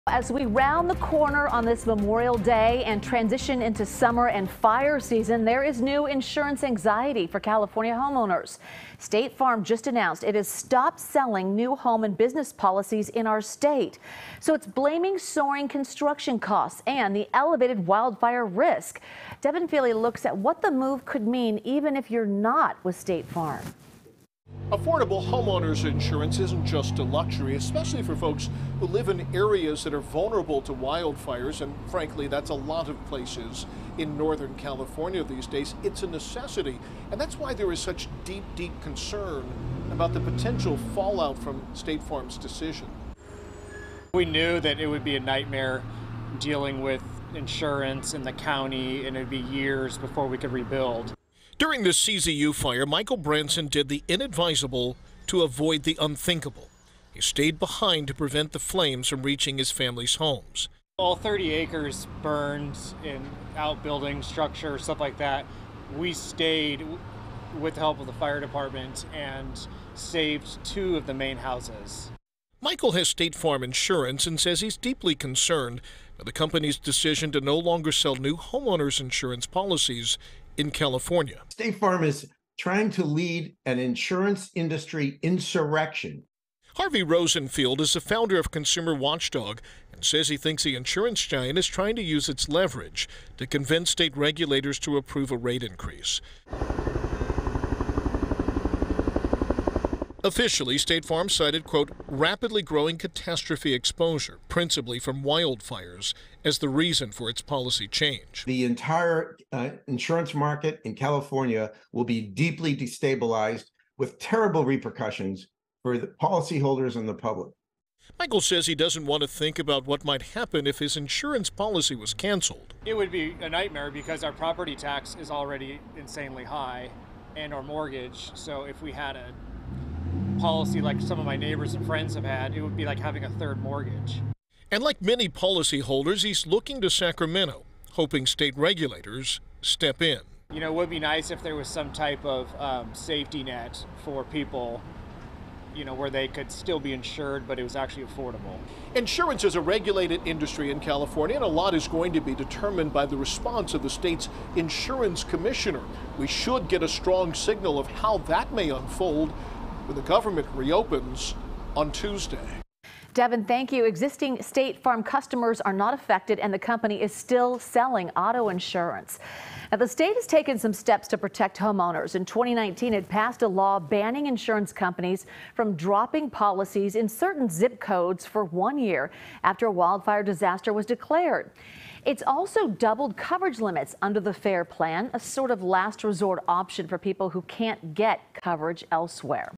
State Farm move to stop providing new home insurance policies in California raises concerns
Devin Fehely reports on California residents worried about State Farm's plan to not offer new homeowner insurance policies in the state (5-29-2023)